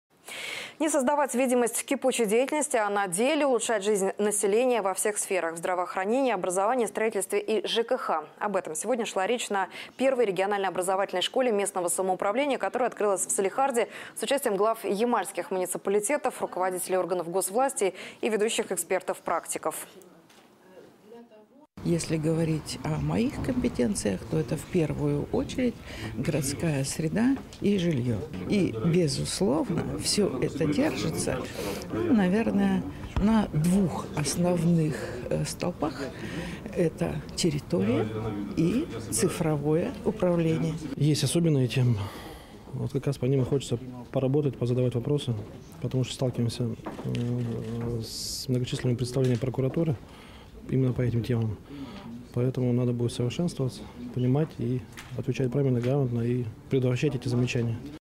Советник РААСН А.Н. Береговских в «Школе местного самоуправления» (Салехард)
Не создавать видимость кипучей деятельности, а на деле улучшать жизнь населения во всех сферах – в здравоохранении, образовании, строительстве и ЖКХ. Об этом сегодня шла речь на первой региональной образовательной школе местного самоуправления, которая открылась в Салехарде с участием глав ямальских муниципалитетов, руководителей органов госвласти и ведущих экспертов-практиков.
Анна Береговских – советник РААСН, член рабочих групп Минэкономразвития: «Если говорить о моих компетенциях, то это в-первую очередь городская среда и жилье. И, безусловно, все это держится на двух основных столпах – это территория и цифровое управление».
Сергей Баранов – глава МО Аксарковское: «Есть особенные темы, по ним и хочется поработать, позадавать вопросы. Сталкиваемся с различными представлениями прокуратуры. Надо будет совершенствоваться, и предотвращать эти замечания».